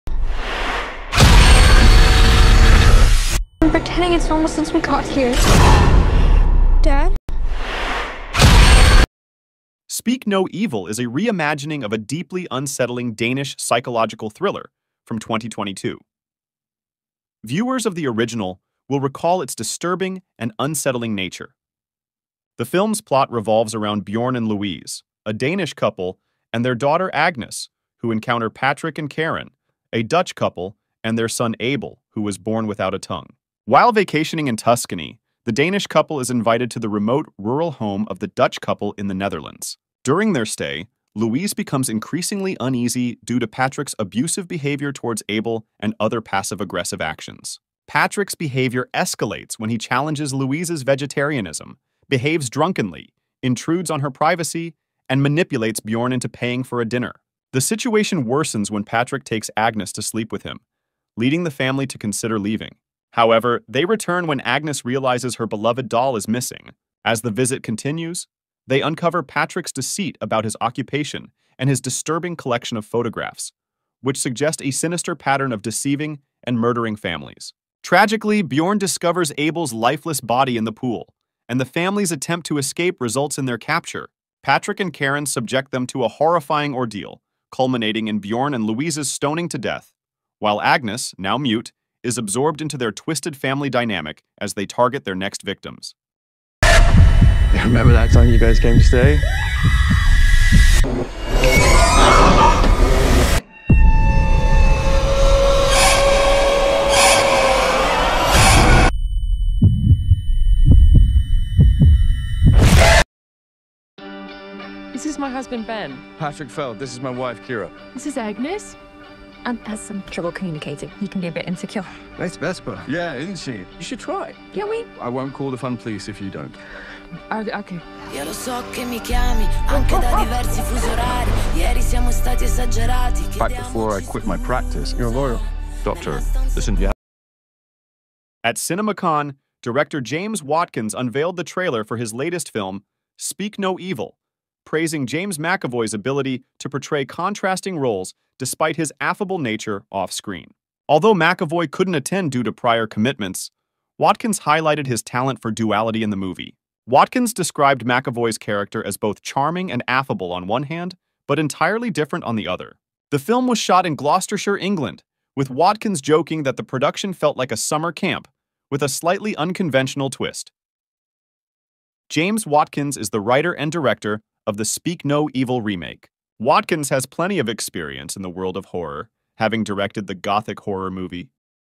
Upcoming Psychological Thriller Movie Speak No Evil Explained
At CinemaCon, director James Watkins unveiled the trailer for his latest film, "Speak No Evil," praising James McAvoy's ability to portray contrasting roles despite his affable nature off-screen. Although McAvoy couldn't attend due to prior commitments, Watkins highlighted his talent for duality in the movie. Watkins described McAvoy's character as both charming and affable on one hand, but entirely different on the other. The film was shot in Gloucestershire, England, with Watkins joking that the production felt like a summer camp with a slightly unconventional twist.